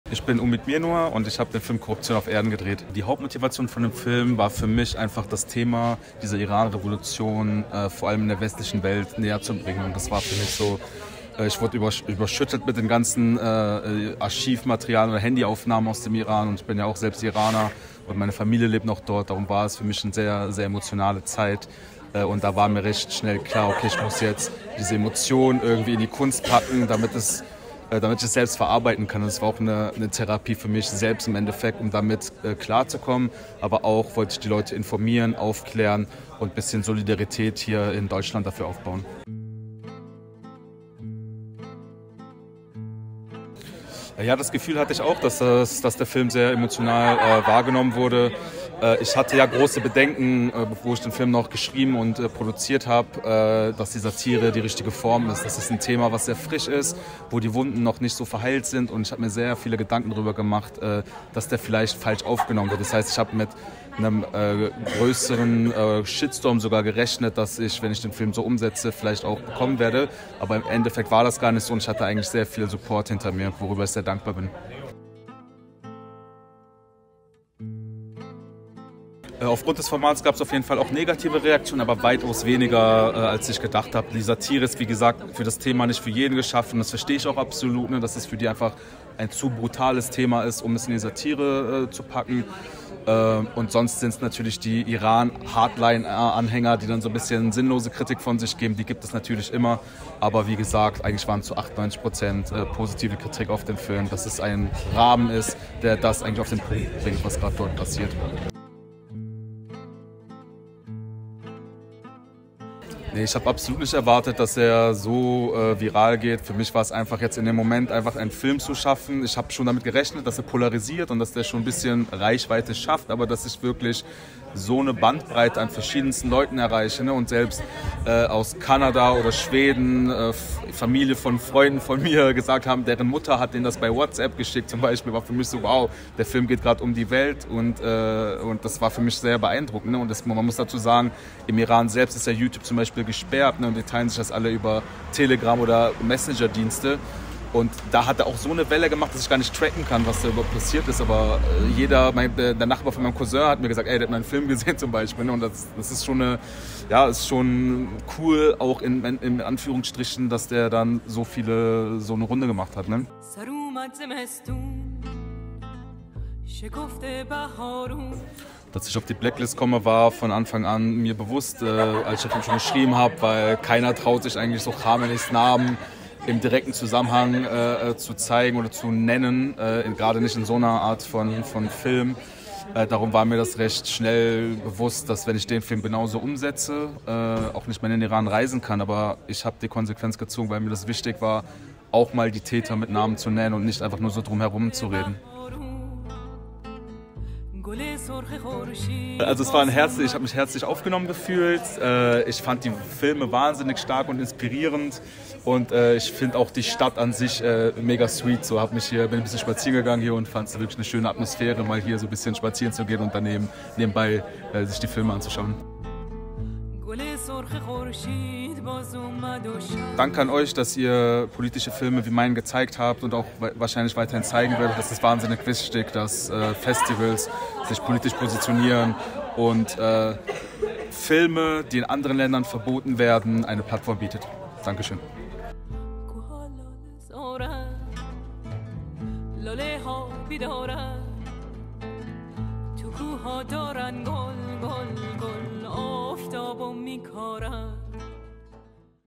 Omid Interview VÖ
In dem Interview erzählt Omid von der schwierigen Situation im Iran, den Ungerechtigkeiten des Regimes und von seinem Kurzfilm, der auf dem Internationalen Kurzfilm Festival in Detmold gezeigt wurde.

Solidarität mit den Menschen im Iran.
In Erinnerung an Jina Mahsa Amini.
Solidarität mit allen regimekritischen Stimmen.
Für den Frieden von uns allen.
Für eine offene Gesellschaft.

Die Revolution hat begonnen.